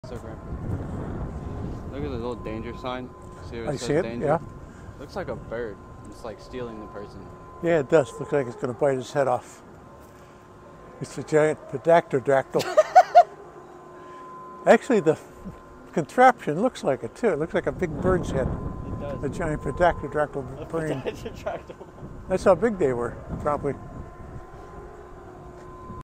Odd Looking Oil Rig 🤣
Oil Rig looks like a huge bird

Turn on notifications for friendly reminders about our upcoming videos.